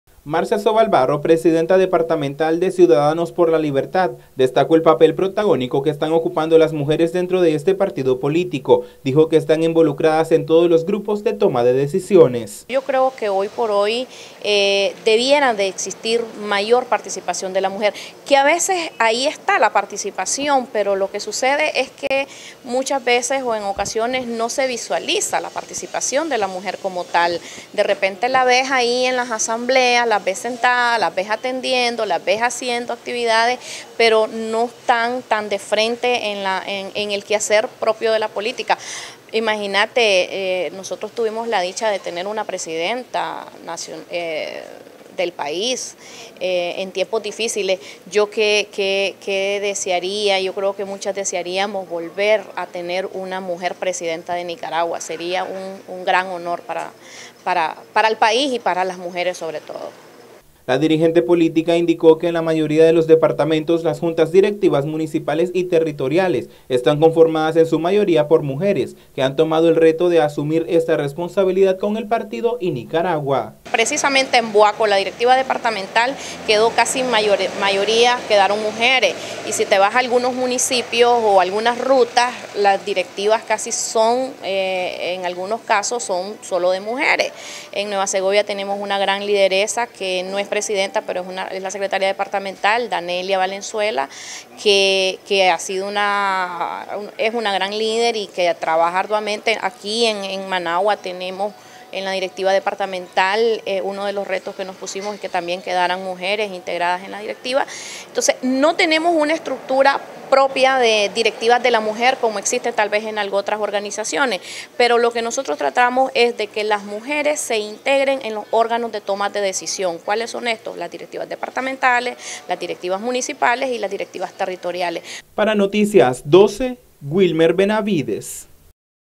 DESTACAN PAPEL DE LA MUJER EN LA POLÍTICA
Las mujeres en la política juegan un papel importante, destacó Marcia Sobalvarro de CxL.